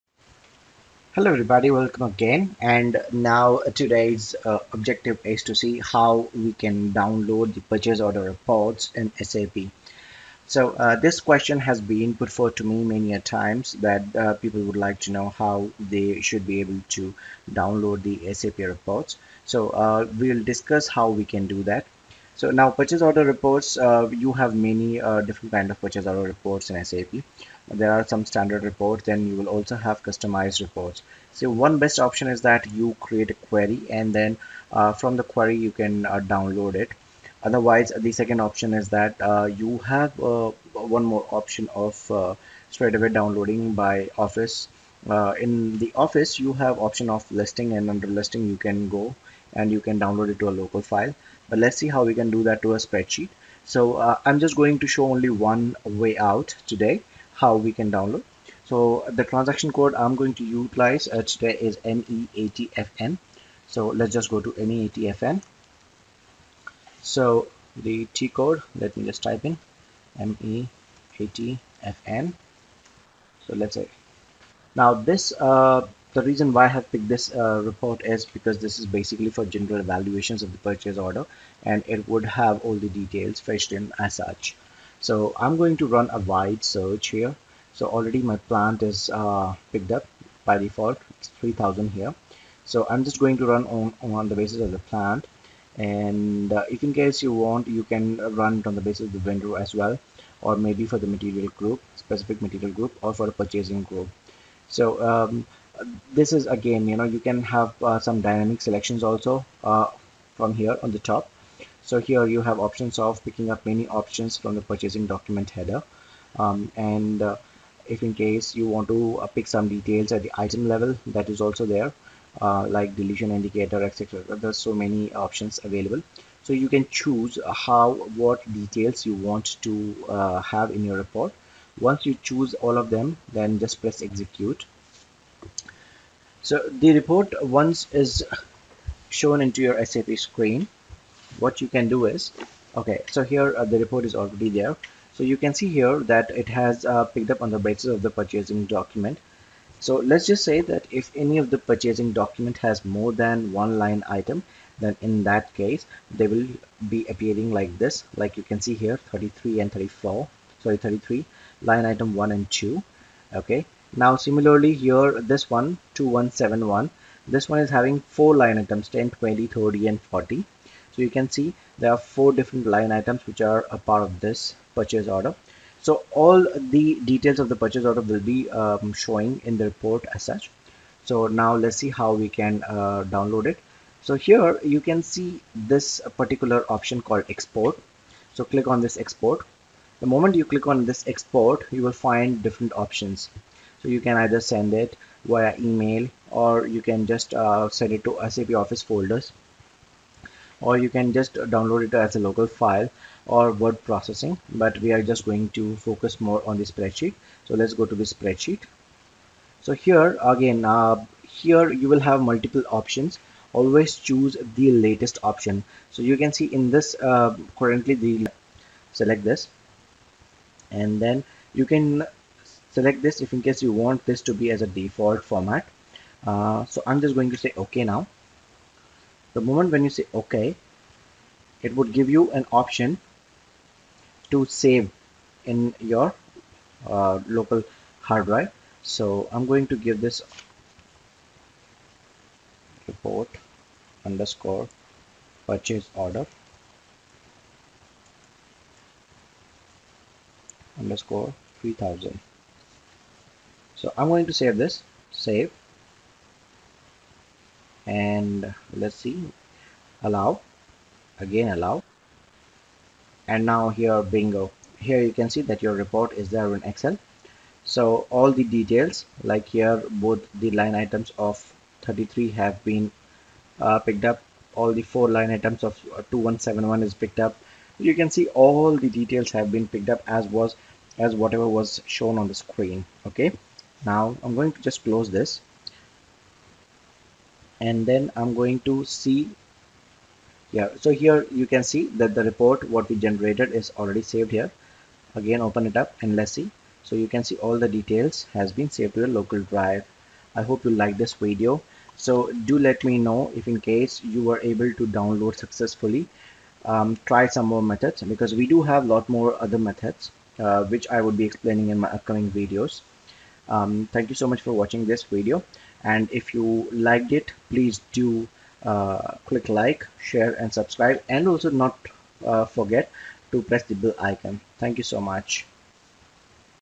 How to SAP Purchase Order report Download(2025)
Learn how to SAP Purchase order report download . Purchase order Reports are very important documents and everyday analysis is a must like Open Purchase order, Rejected Purchase order, purchase order receipt report etc.